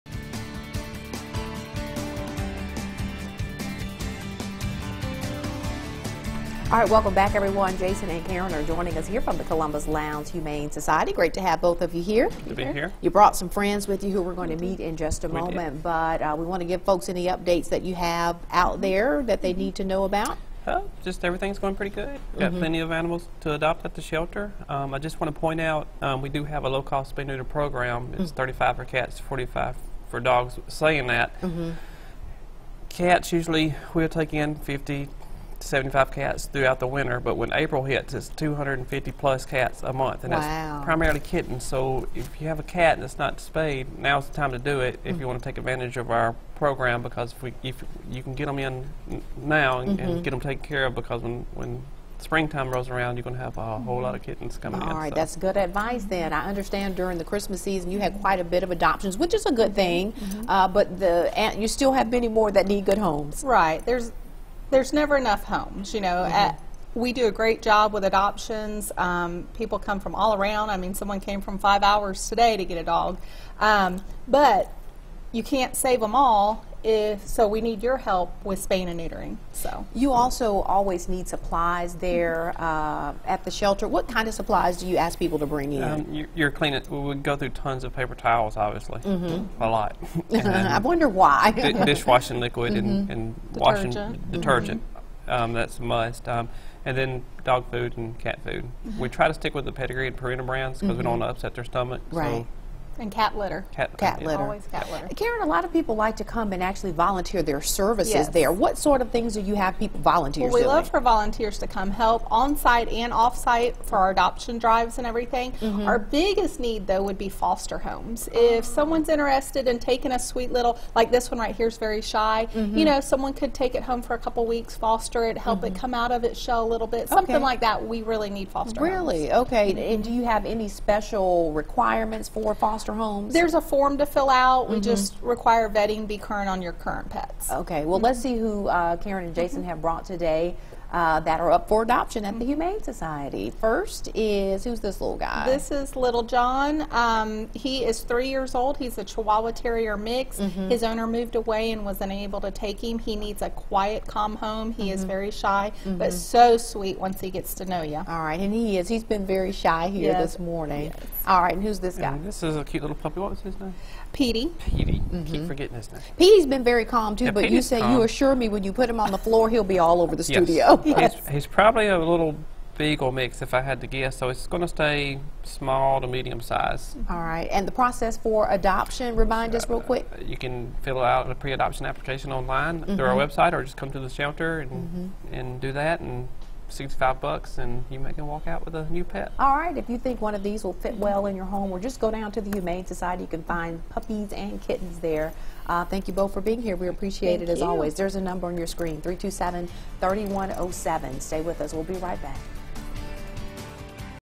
Lowndes County Humane Society
Jason and Karen from the Lowndes County Humane Society joins us to talk about adoption.